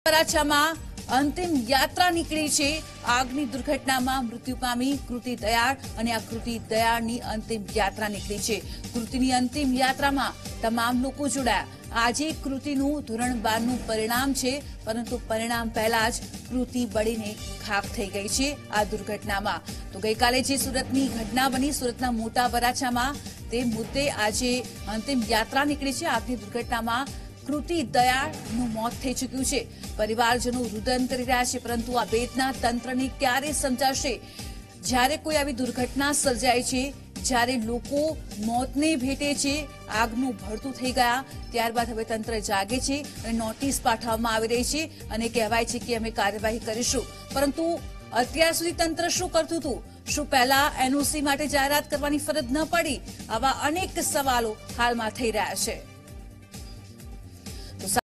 Last ritual of Surat fire victim Kruti dayal
સુરતના સરથાણા વિસ્તારના તક્ષશિલા આર્કેડમાં ગઇકાલે બનેલી આગની દુર્ઘટનામાં 20 વિદ્યાર્થીઓના મોત થયા છે. આ કમખ્વાર ઘટનામાં સુરતના મોટા વરાછા ખાતે આવેલા ક્રિષ્નાપાર્ક ખાતે રહેતી કૃતિ નિલેશભાઈ દયાળનું મૃત્યુ થયું છે. કૃતિની અંતિમયાત્રામાં મોટી સંખ્યામાં લોકો જોડાયા હતા. આજે કૃતિનું ધોરણ 12નું પરિણામ હતું પણ તે પરિણામ જોઇ શકે તે પહેલાં તેણે અંતિમ વાટ પકડી હતી.